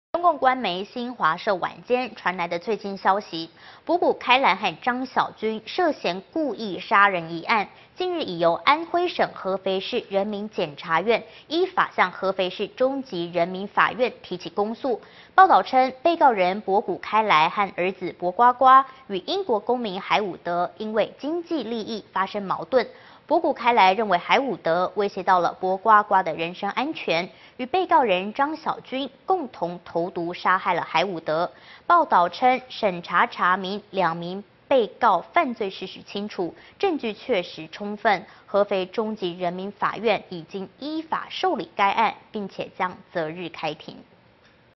【薄谷開來_中国热点真相最新新闻】薄谷开来、张晓军以谋杀罪被起诉
【薄谷開來_中国热点真相最新新闻】薄谷开来、张晓军以谋杀罪被起诉(ChinaBreakingNewsCN)

【新唐人亚太台2012年7月26日讯】中共官媒新华社晚间最新消息。薄谷开来、张晓军涉嫌故意杀人一案，近日已由安徽省合肥市人民检察院依法向合肥市中级人民法院提起公诉。报导称，被告人薄谷开来和儿子薄瓜瓜，与英国公民海伍德因经济利益发生矛盾，薄谷开来认为海伍德威胁到了薄瓜瓜的人身安全，与被告人张晓军共同投毒杀害了海伍德。报导称审查查明，两名被告犯罪事实清楚，证据确实、充分。合肥中级人民法院已依法受理该案并将择日开庭。

(NTDAPTVNewsCN)